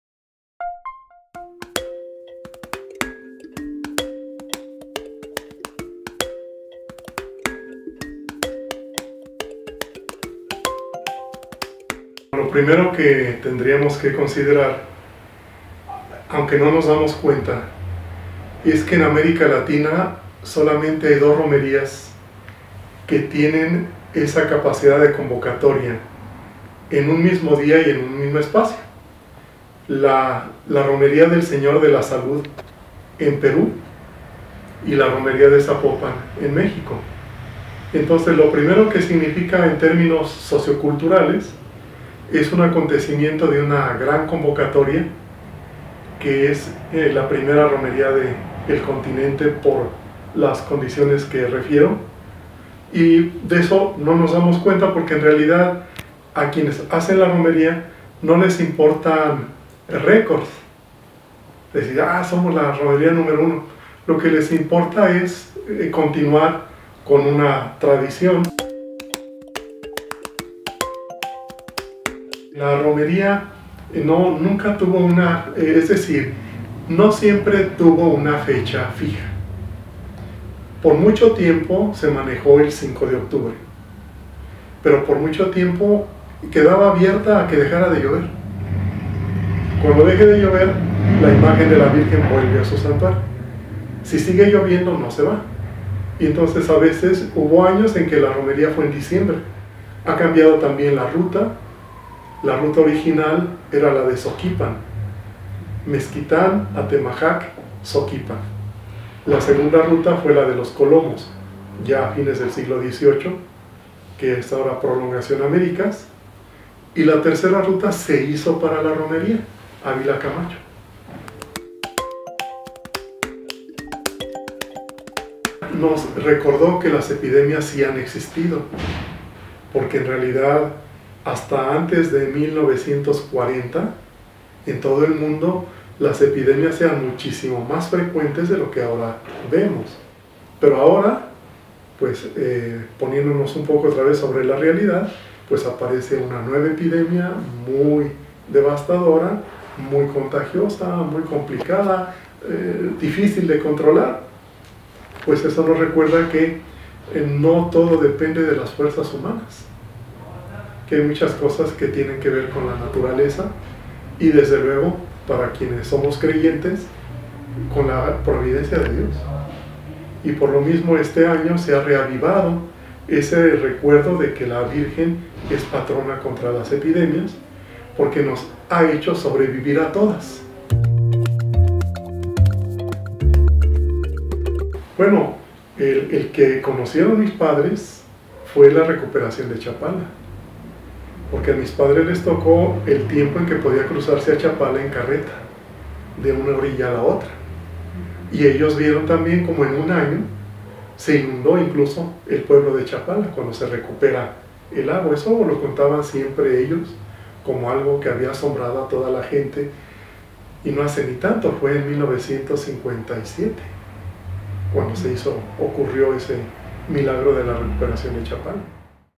Cambia coronavirus tradición de Romería
Explica Arquidiócesis importancia de la procesión para los tapatíos.